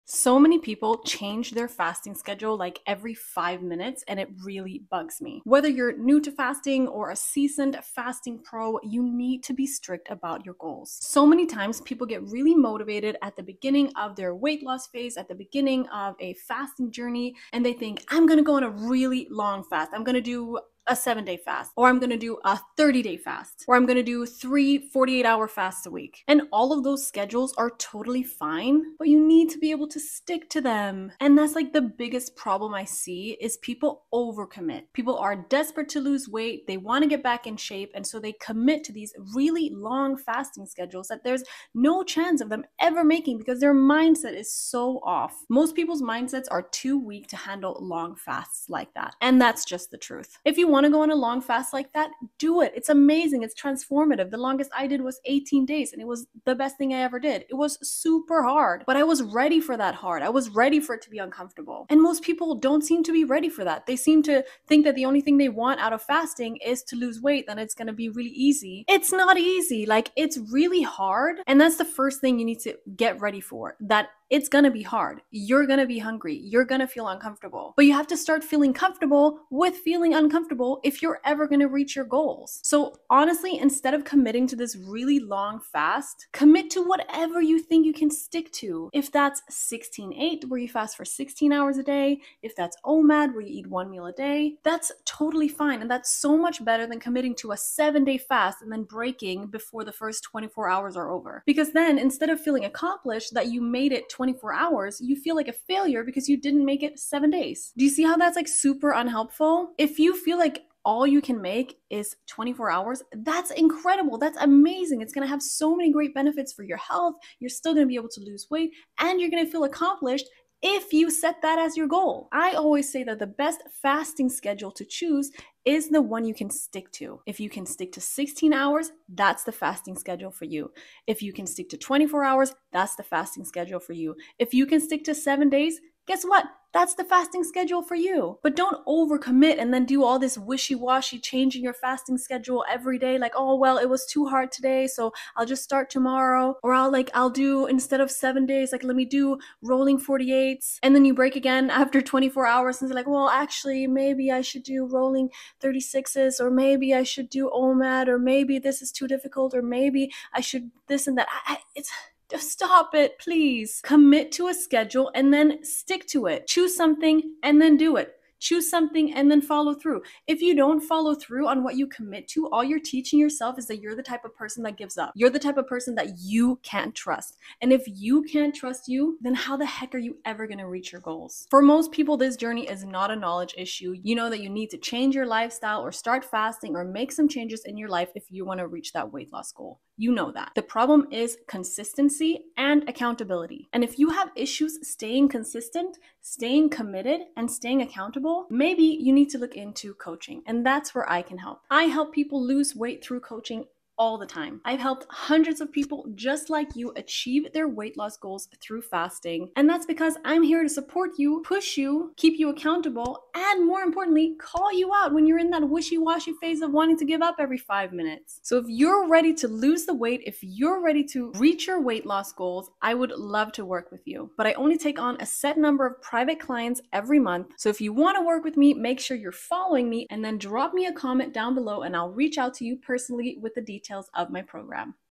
Stick to one schedule
You should stick to one fasting schedule!